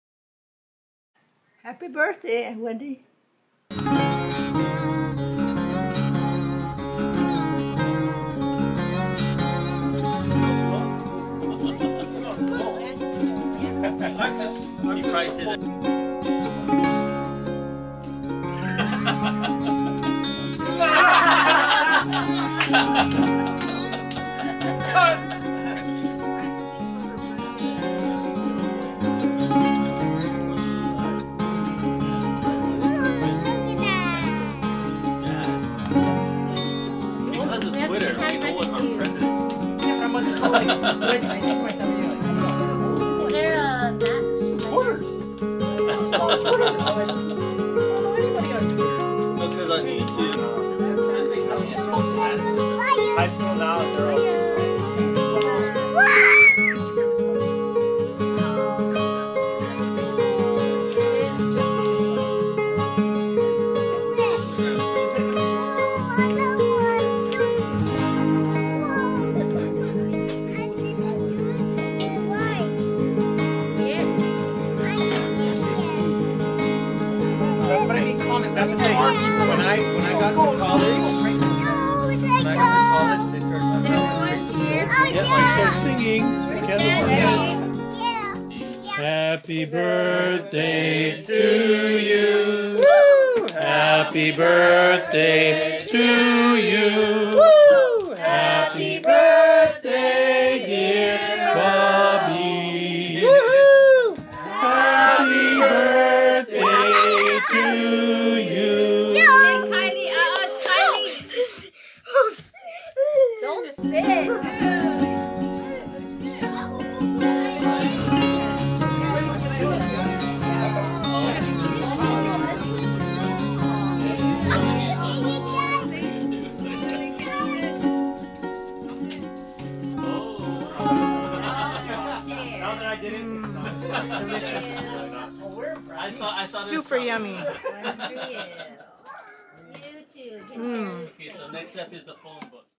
Birthday, with a ZV-1
Here is a Birthday, cine 2, corrected in Premier,  forgot to turn ND off indoors.